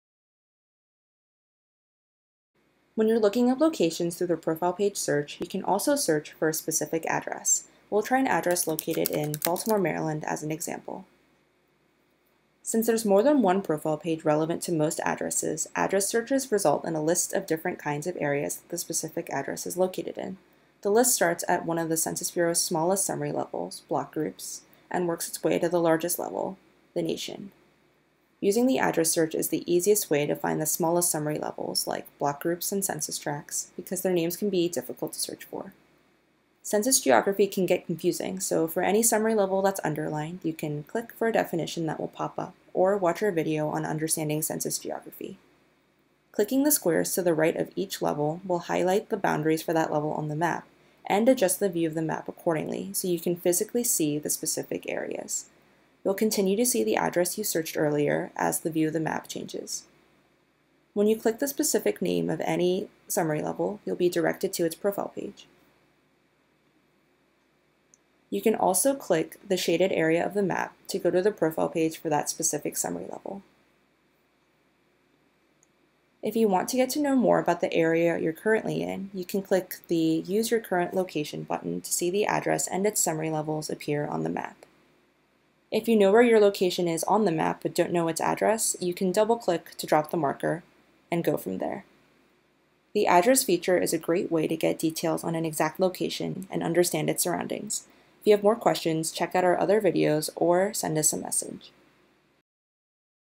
Using Address Search on Census Reporter
Watch this video to learn about Census Reporter's powerful address search feature, which can help you identify all the geographies relevant to any street address.